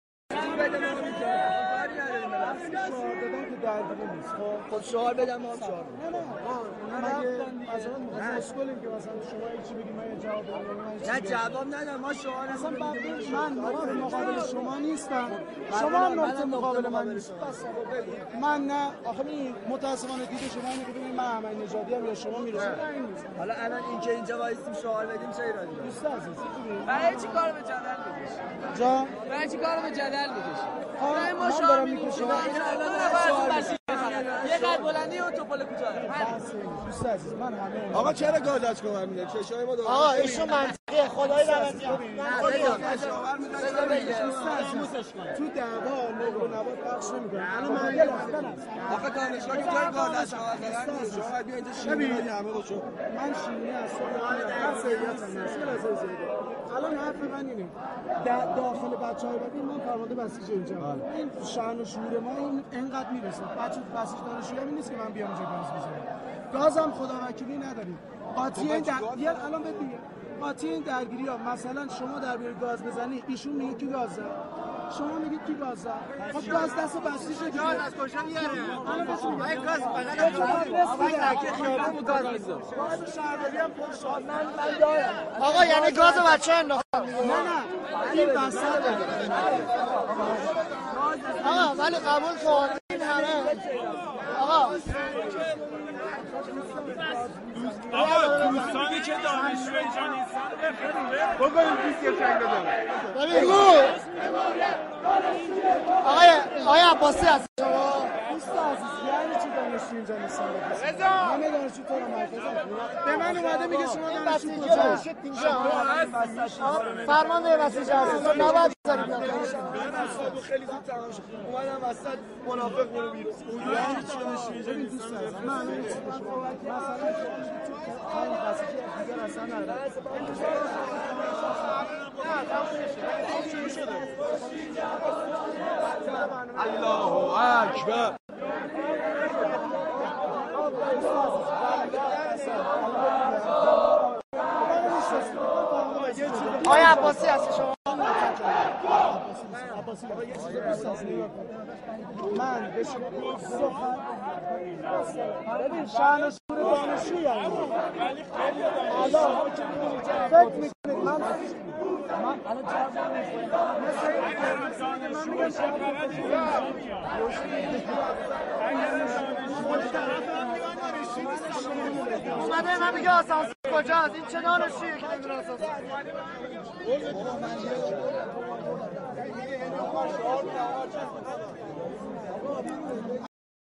بحث بسیجج دانشگاه با دانشجویان دانشگاه آزاد Iran tehran
This is the leader of the Basiji at Tehran Azad University Central branch, which are argueeing with the student!!

Students ask him why they has throwen tear gas at the students and also why are the so many no student basiji at the university!!

Tehran Azad Uni 13 Oct